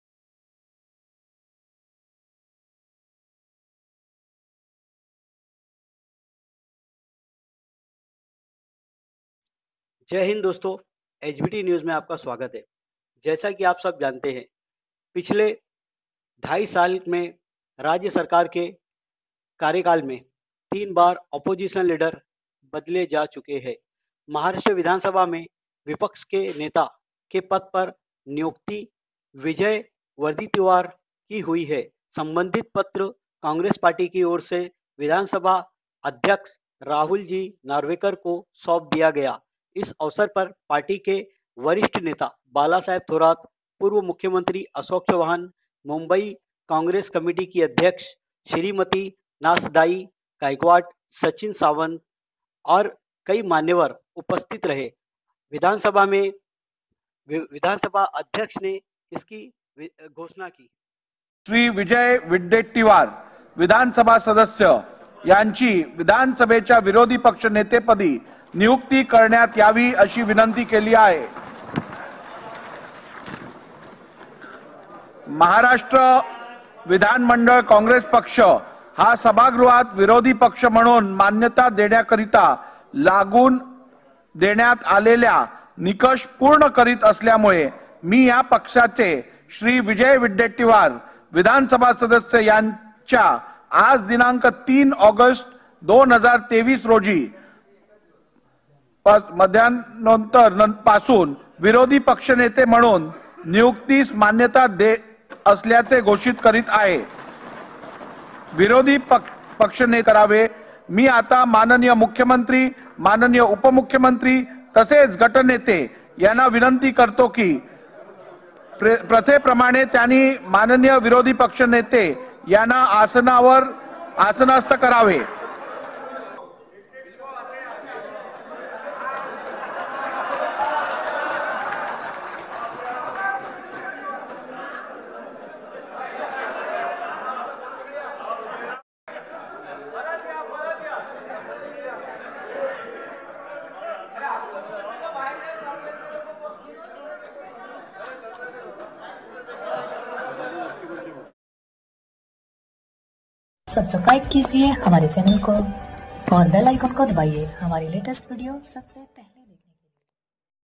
राज्य सरकार के ढाई साल के कार्यकाल में तीन बार बदले Opposition Leader | Hindbhumi Times
मुंबई | यूसुफ मंसूरी

महाराष्ट्र राज्य विधानसभा में विपक्ष के नेता के पद पर नियुक्ति विजय वाडेटीवार की हुई संबंधित पत्र कांग्रेस पार्टी की ओर से विधानसभा अध्यक्ष राहुलजी नार्वेकर को सौंप दिया गया। इस अवसर पर पार्टी के वरिष्ठ नेता बालासाहेब थोराट, पूर्व मुख्यमंत्री अशोक चव्हाण, मुंबई कांग्रेस कमेटी की अध्यक्ष श्रीमती. नाशदाई गायकवाड, सचिन सावंत एवं अन्य गणमान्य लोग उपस्थित थे